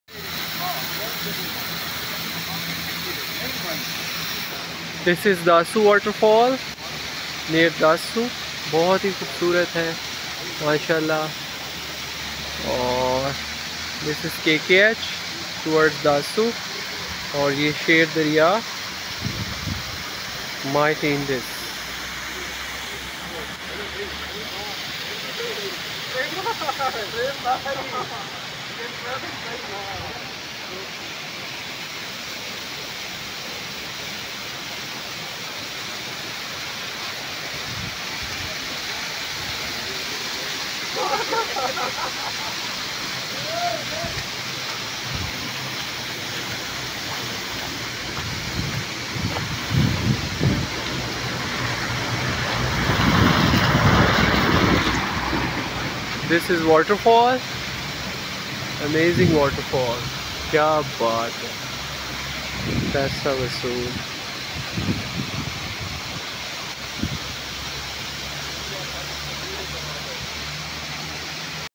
Dassu Aabsharain, KKH Ka Nasha
Another Vlog About Dassu Waterfalls On KKH With Indus River.